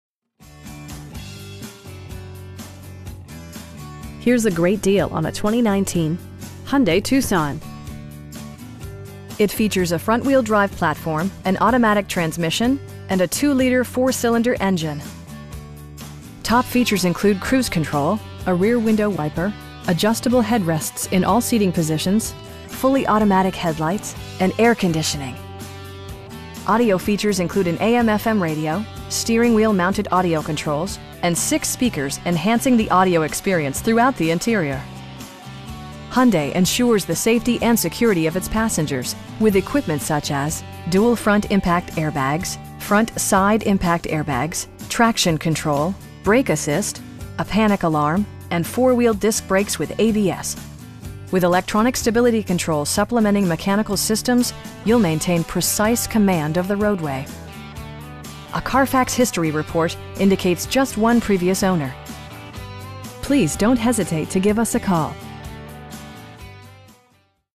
2019 Hyundai Tucson SE in Madison, TN 37115
Town & Country
101 Anderson Lane

Here's a great deal on a 2019 Hyundai Tucson. It features a front-wheel-drive platform, an automatic transmission, and a 2 liter 4 cylinder engine. Top features include cruise control, a rear window wiper, adjustable headrests in all seating positions, front bucket seats, fully automatic headlights, power door mirrors, an overhead console, and air conditioning. Audio features include an AM/FM radio, steering wheel mounted audio controls, and 6 speakers, enhancing the audio experience throughout the interior. Hyundai ensures the safety and security of its passengers with equipment such as: dual front impact airbags, front side impact airbags, traction control, brake assist, a panic alarm, and 4 wheel disc brakes with ABS. With electronic stability control supplementing mechanical systems, you'll maintain precise command of the roadway. A Carfax history report indicates just one previous owner. We pride ourselves on providing excellent customer service. Please don't hesitate to give us a call.